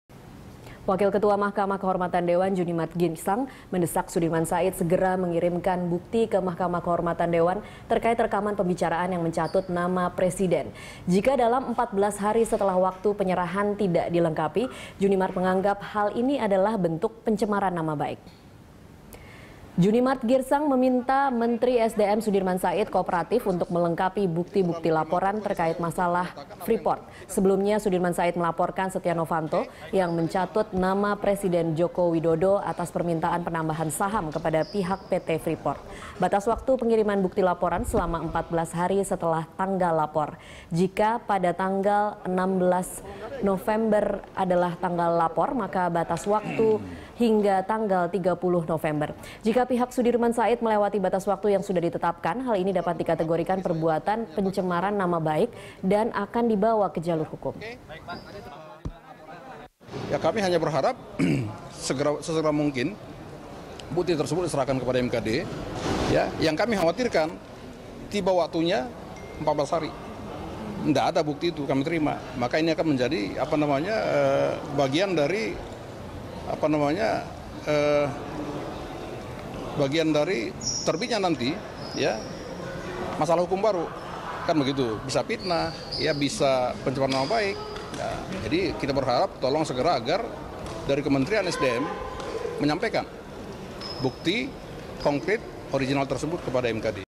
MKD Desak Menteri Sudirman Serahkan Bukti Rekaman
Wakil Ketua Mahkamah Kehormatan Dewan (MKD) Junimart Girsang mendesak Menteri ESDM Sudirman Said segera mengirimkan bukti rekaman pembicaraan yang mencatut nama Presiden.

Facebook.com/BeritaSatuTV
Youtube.com/BeritaSatu